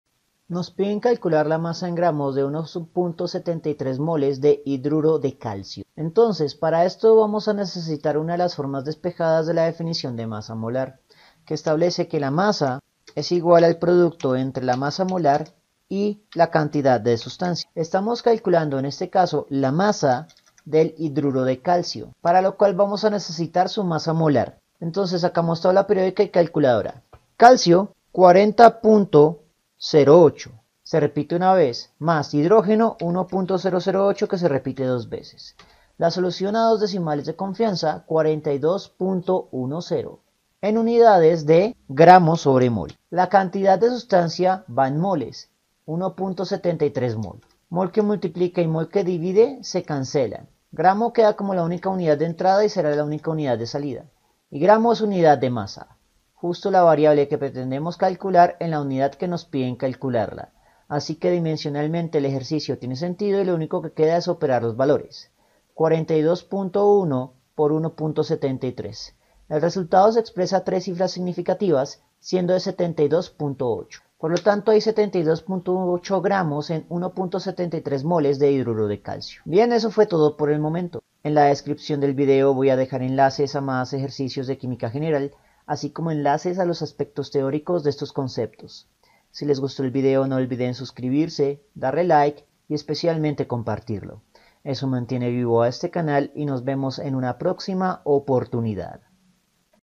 Como 𝐜𝐚𝐥𝐜𝐮𝐥𝐚𝐫 la 𝐦𝐚𝐬𝐚 en 𝐠𝐫𝐚𝐦𝐨𝐬 de 1.73 𝐦𝐨𝐥𝐞𝐬 de 𝐂𝐚𝐇𝟐
👉 𝑬𝒏𝒖𝒏𝒄𝒊𝒂𝒅𝒐: Calcular la masa en gramos de 1.73 mol de CaH2.

👉 𝑺𝒐𝒍𝒖𝒄𝒊ó𝒏: 72.8 g